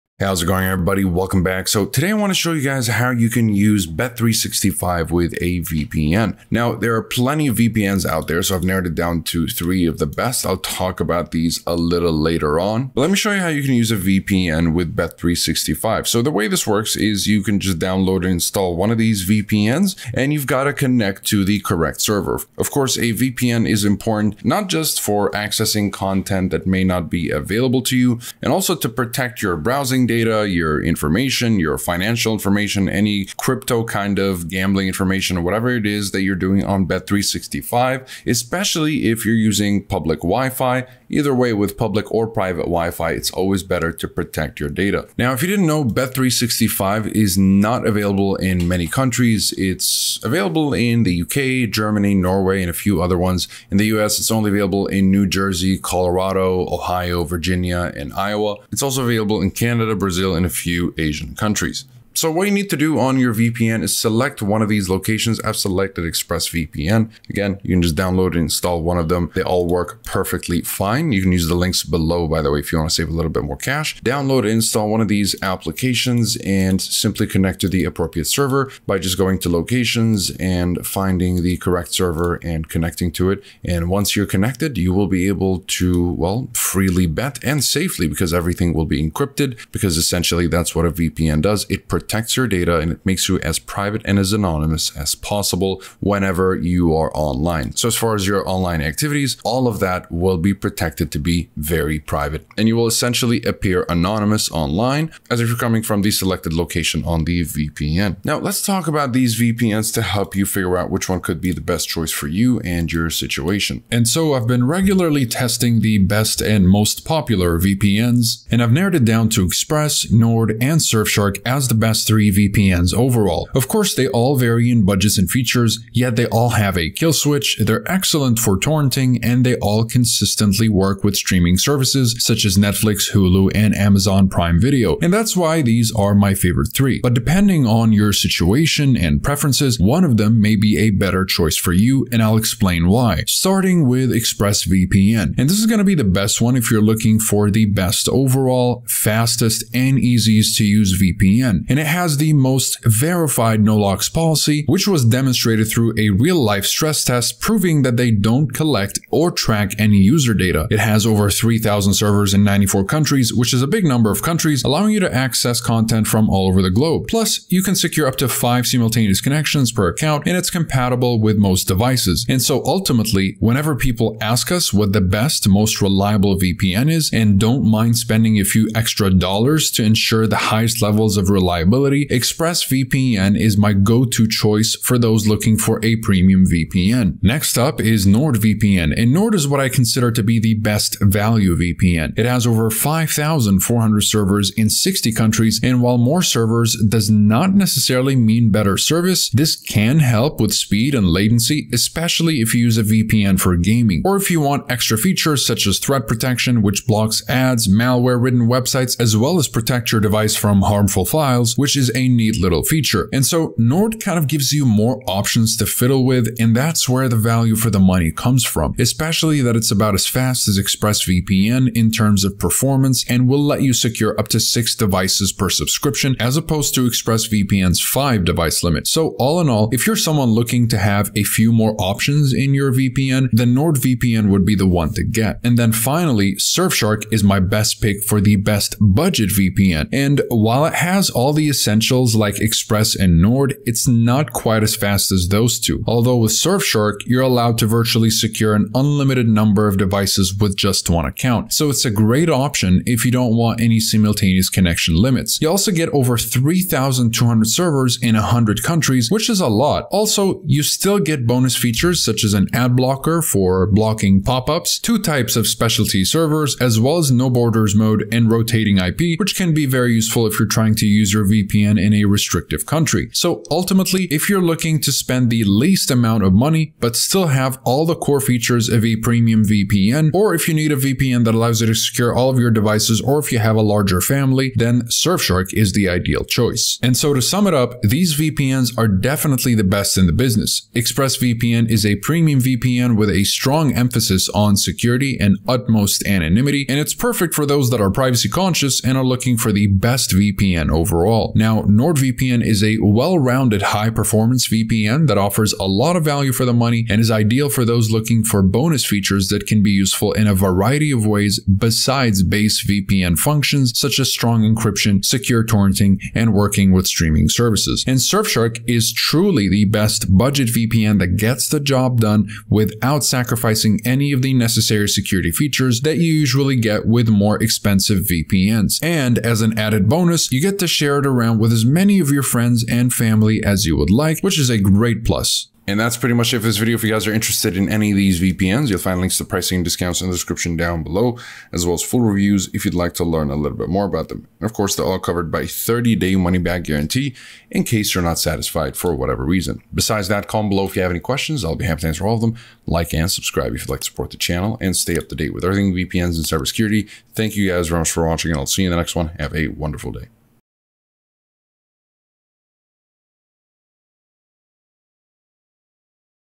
How to Use Bet365 With a VPN
How to Use Bet365 With a VPN. Bet on Bet365 with ease using a VPN! Learn how to access and enjoy online gambling securely from anywhere.
💣 SPECIAL VPN DISCOUNTS ARE FOUND IN PINNED COMMENT 💣

How's it going, everybody? Welcome back. So today I want to show you guys how you can use BIM 365 with a VPN. Now, there are plenty of VPNs out there. So I've narrowed it down to three of the best, I'll talk about these a little later on, but let me show you how you can use a VPN with BIM 365. So the way this works is you can just download install one of these VPNs, and you've got to connect to the correct server. Of course, a VPN is important, not just for accessing content that may not be available to you, and also to protect your browsing data, your information, your financial information, any crypto kind of gambling information, or whatever it is that you're doing on Bim 365, especially if you're using public Wi Fi, either way, with public or private Wi Fi, it's always better to protect your data. Now, if you didn't know Bim 365 is not available in many countries. It's available in the UK, Germany, Norway, and a few other ones. By the way, if you want to save a little bit more cash Download, install one of these applications and simply connect to the appropriate server by just going to locations and finding the correct server and connecting to it. And once you're connected, you will be able to well freely bet and safely because everything will be encrypted, because essentially, that's what a VPN does. It protects your data. And it makes you as private and as anonymous as possible whenever you are online. So as far as your online activities, all of that will be protected to be very private, and you will essentially appear anonymous online as if you're coming from the selected location on the VPN. Now let's talk about these VPNs to help you figure out which one could be the best choice for you and your situation. And working with streaming services and surf shark is truly the best budget VPN that gets the job done without sacrificing any of the necessary security features that usually get with more expensive VPNs. And as an added bonus, you get to share it around with as many of your friends and family as you would like, which is a great plus. And that's pretty much it for this video. If you guys are interested in any of these VPNs you'll find links to pricing discounts in the description down below, as well as full reviews if you'd like to learn a little bit more about them. Of course they're all covered by 30 day money back guarantee. In case you're not satisfied for whatever reason besides that calm below if you have any questions, I'll be happy to answer all of them. Have a wonderful day.

Hope you enjoyed my How to Use Bet365 With a VPN Video.